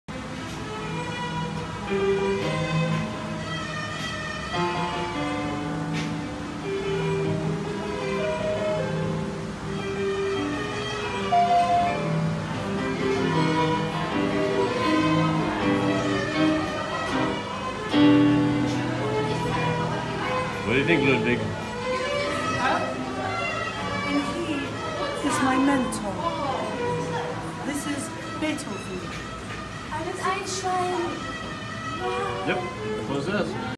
Zümi tickling Beethoven’s keys.. in public!
While at Madame Tussaud’s, Zümi decides to show Beethoven how it’s done... Zümi style!